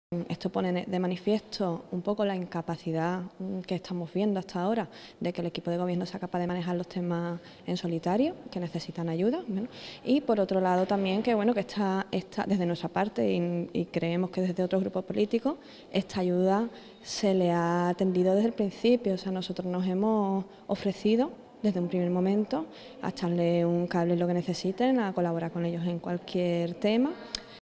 Mano tendida de Ciudadanos que cuestiona la capacidad de gobierno en el Ayuntamiento Cádiz
El grupo municipal, que cuenta con dos concejales en la corporación local, ofrece su colaboración para alcanzar puntos de consenso